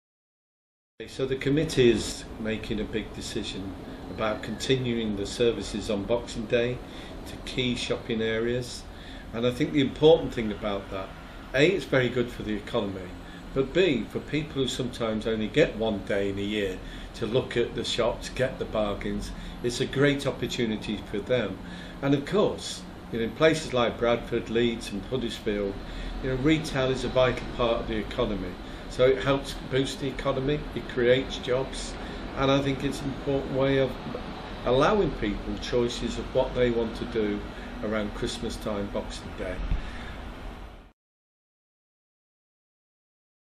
Boxing Day Bus Services for West Yorkshire 2015
Councillor Keith Wakefield, Chair of West Yorkshire Combined Authority's Transport Committee talks about Boxing Day Bus services